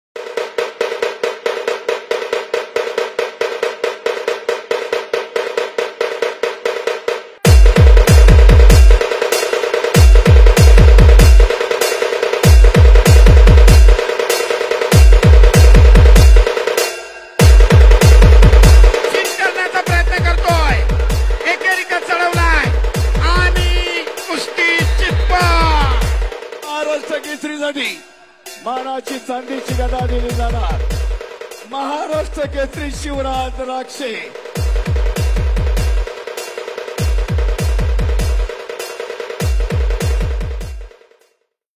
Top moments from the Grand Finale of Maharashtra Kesari 2023 l Pune l Shivraj Rakshe
Exciting moments from the Grand Finale of Maharashtra Kesari 2023 l Pune l Shivraj Rakshe#eknathshinde